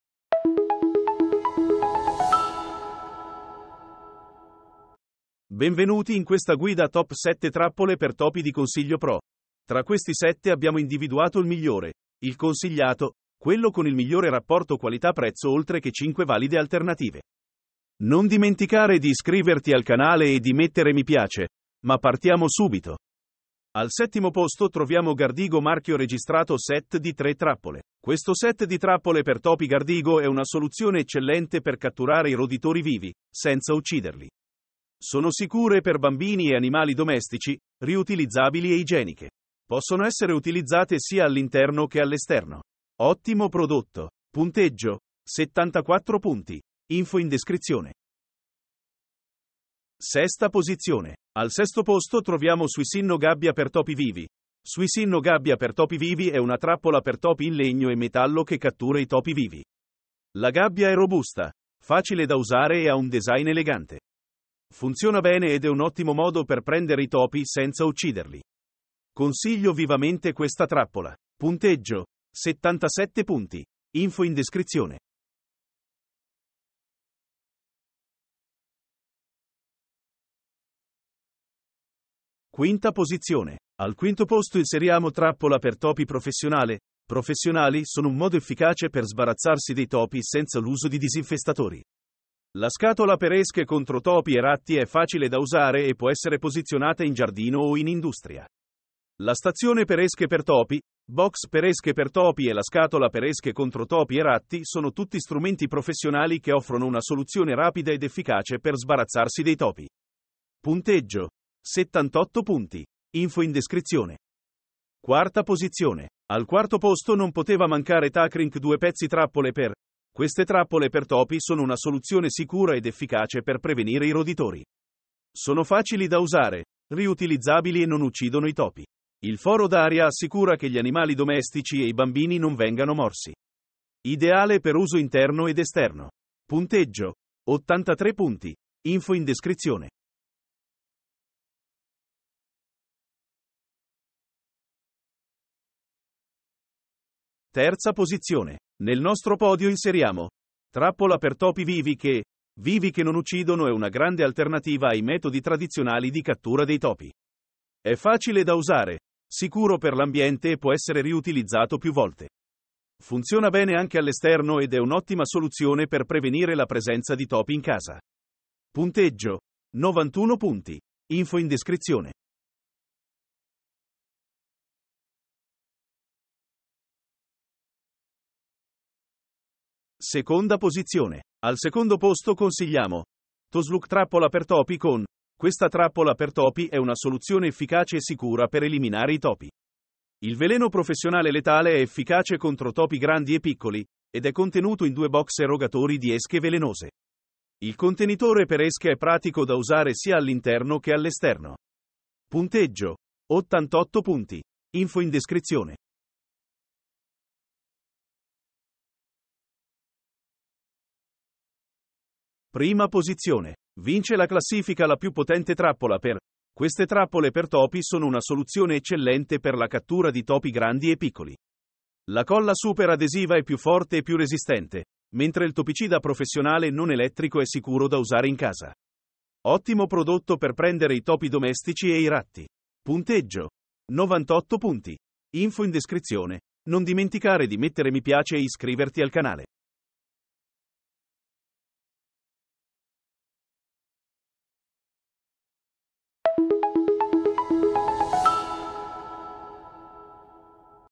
Migliori trappole per topi Top 7 del 2023
🔥 Nome Articolo: Le 6 Migliori Trappole per topi - Le Ultra efficaci che funzionano!
Stai cercando una trappola per topi? La guida alla scelta delle migliori trappole per topi ultra efficaci. Le Top 6 con trucchi e Consigli!
👉 Per vedere i prezzi e altre foto, ecco i link qui sotto
Top sette migliori trappole per topi del 2023 di ConsiglioPro.it
► 1° in classifica La Più Potente Trappola per
► 2° in classifica TOSLUC Trappola per Topi con
► 3° in classifica Trappola per Topi Vivi che
► 4° in classifica TAKRINK 2 Pezzi Trappole per
► 5° in classifica: Trappola per topi professionale
► 6° in classifica SWISSINNO Gabbia per topi vivi
► 7° in classifica: Gardigo® Set di 3 Trappole

Hai bisogno di un consiglio personale? Commenta o visita il nostro sito

⏱🚨🚛 ⎮Prova Prime Gratis 30 gg (Spedizione gratuita e CONSEGNA IN 24 ORE!
Come? Qui